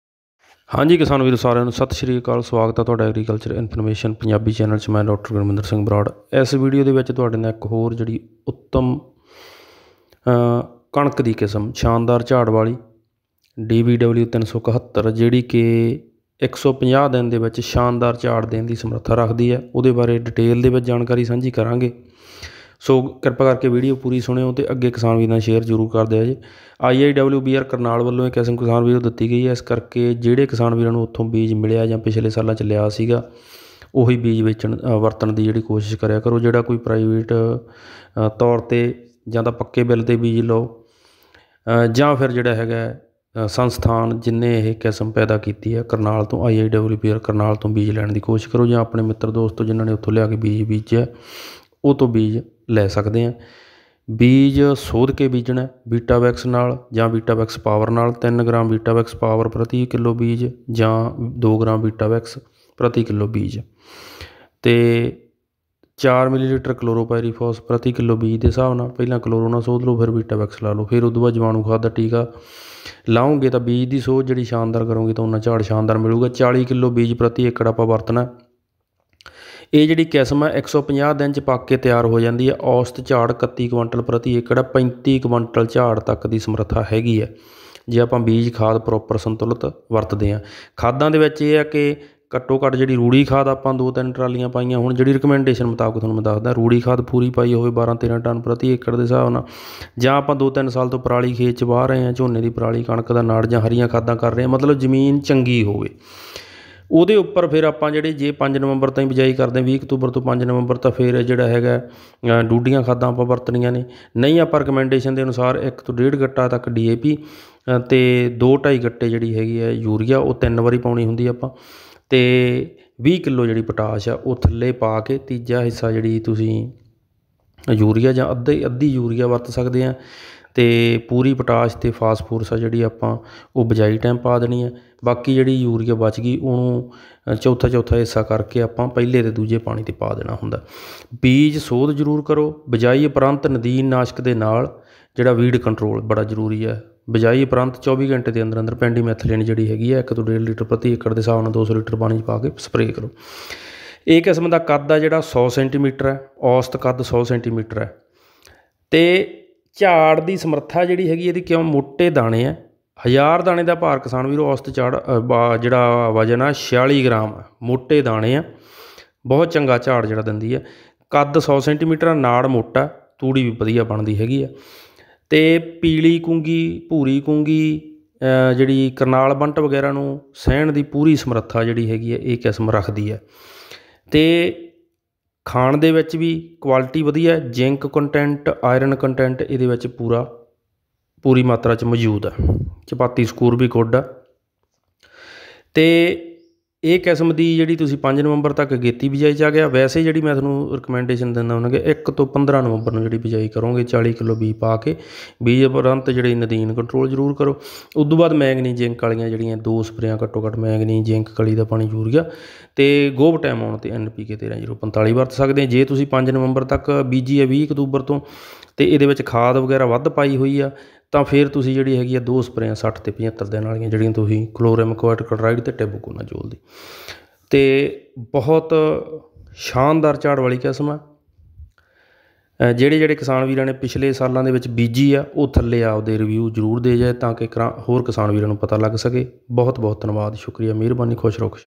ਕਣਕ DBW 371 ਕਿਸਮ 150 ਦਿਨ ਚ 35 ਕੁਇੰਟਲ ਤੱਕ ਝਾੜ ਦੀ ਸਮਰੱਥਾ | DBW 371 variety details
ਬੀਜ ਸੋਧ ਅਤੇ ਛੋਟੇ ਤੱਤ ਖਾਸਕਰ ਜਿੰਕ ਮੈਂਗਨੀਜ ਦੀਆਂ ਸਪਰੇਆਂ ਬਹੁਤ ਜਰੂਰੀ